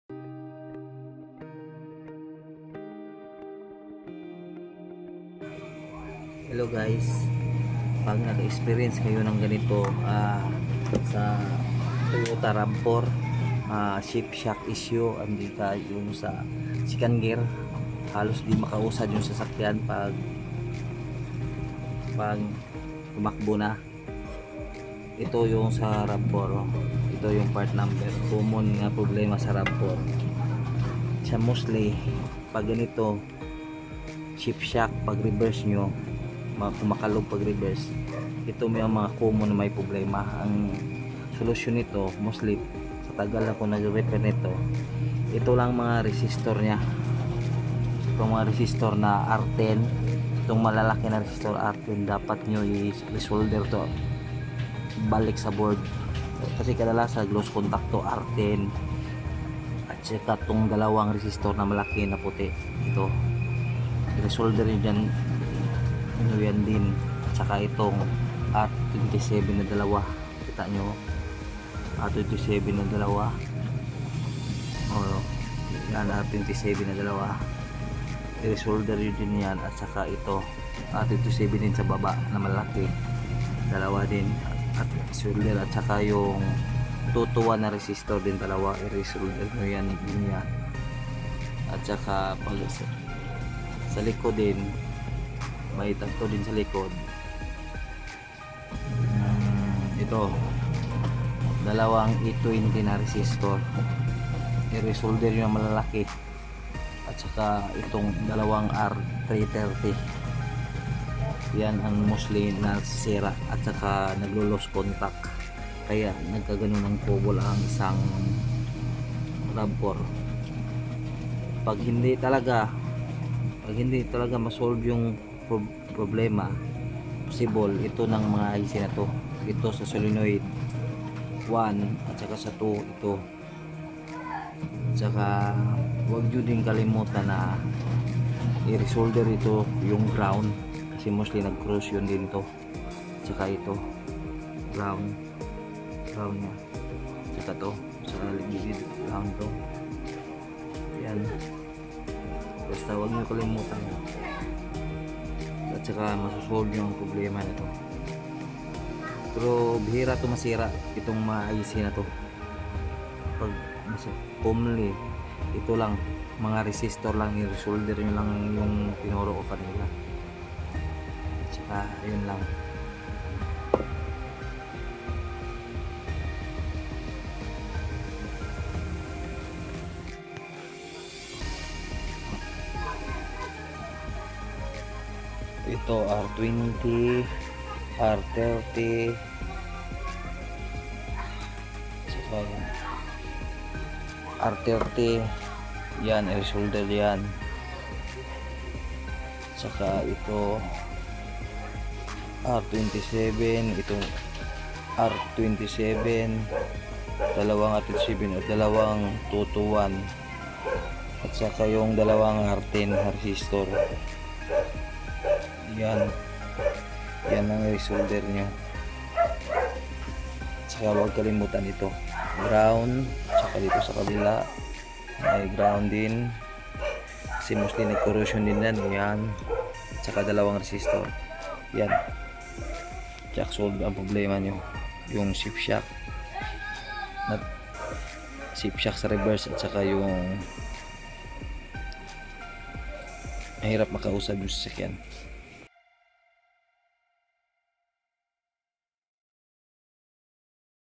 Toyota Rav4 Ecu/Ecm How To Repair Transmission Issue Shift Shock on Reverse
Shift Shock sa Reverse
Harsh Shifting
Resolder Resistor ang Solenoid Driver Ic